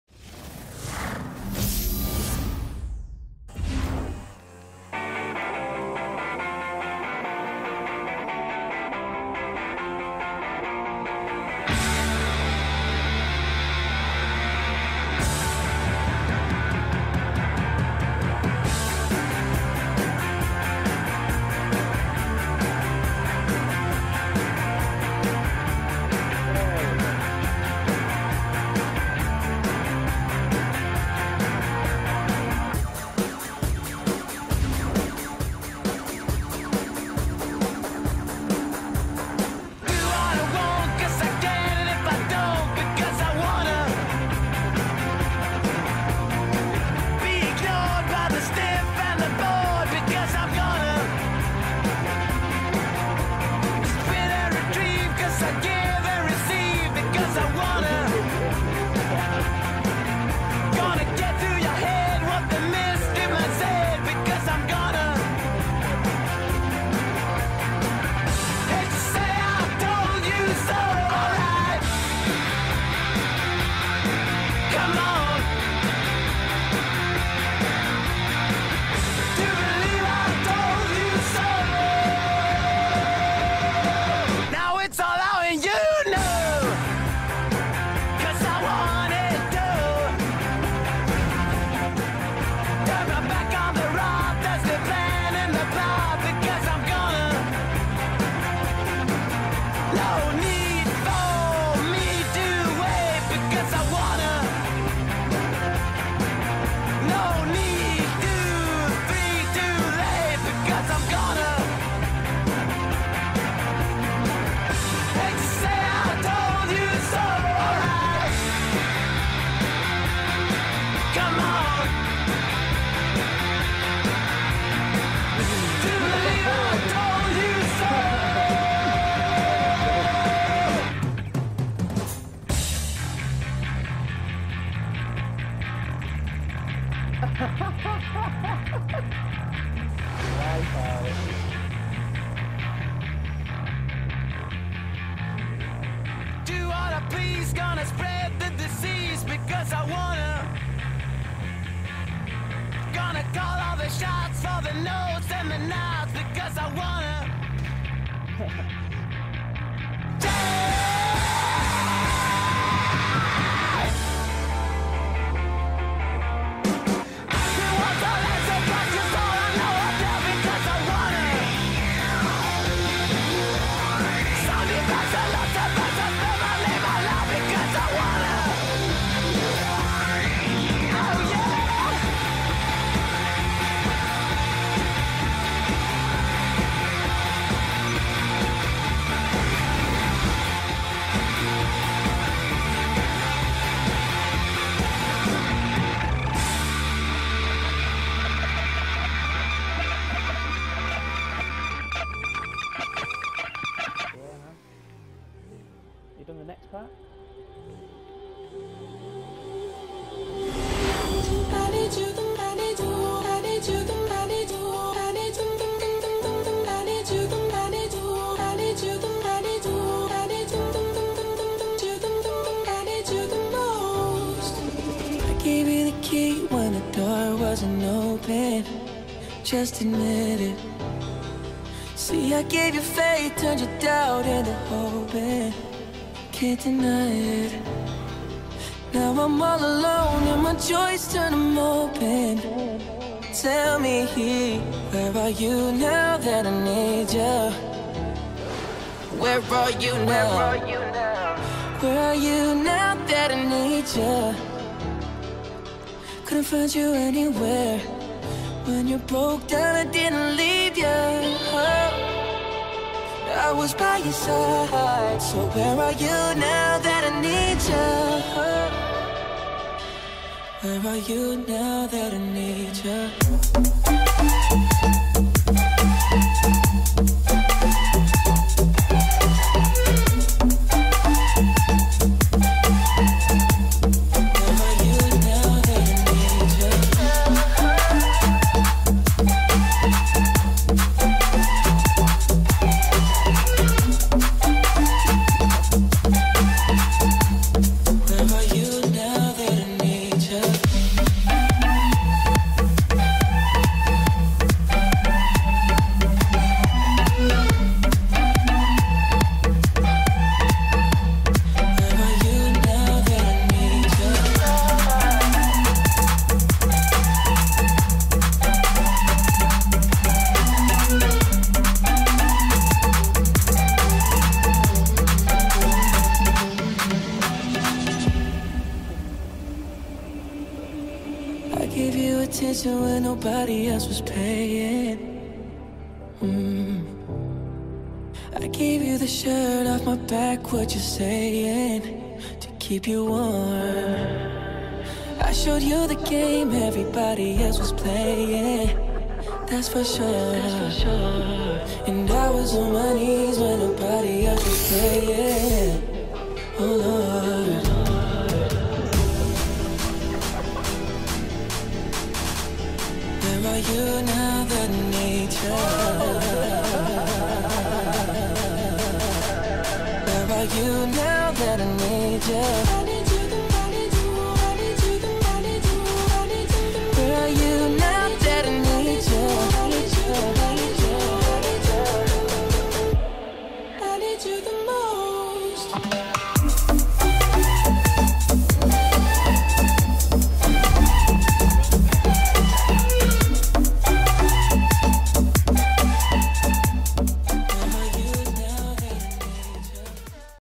Reddit Dad's Stunt Sesh (Wed 20th May)
Just messing around free roam stunting with the Reddit Dads.